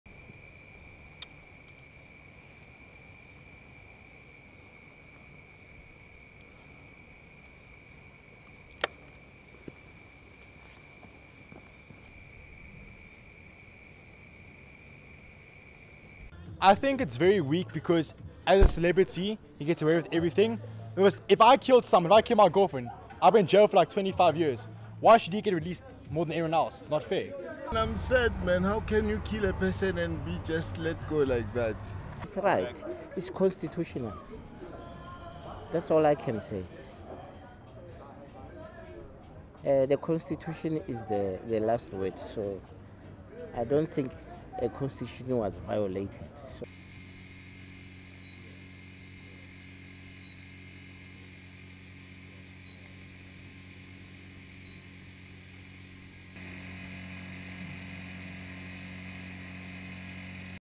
Olympic runner released; under house arrest
(20 Oct 2015) There was mixed public reaction in Pretoria to Oscar Pistorius, the double-amputee Olympic runner who fatally shot his girlfriend in 2013, being released from prison and placed under house arrest on Monday night.
A South African official confirmed the release on Monday.
The Correctional Services Department had originally said Pistorius would be released from the Pretoria jail on Tuesday in line with a decision by a parole board.
Under South African law, an offender sentenced to five years or less in jail can be released after serving one-sixth of the term - in Pistorius' case 10 months.
Pistorius was acquitted of murder last year for the Valentine's Day shooting death of girlfriend Reeva Steenkamp, but prosecutors have appealed the trial verdict of culpable homicide, or manslaughter, and will seek a murder conviction again at South Africa's Supreme Court on 3 November.
If Pistorius is convicted of murder by a panel of five judges at the appeal, he faces going back to prison for 15 years, the minimum sentence for murder in South Africa, which no longer has the death penalty.
While out on bail during his trial, the 28-year-old Pistorius had stayed at his uncle's mansion in an upmarket suburb of Pretoria.
However, an Associated Press journalist outside the house said no one had gone in or out of the main entrance of the house on Monday night.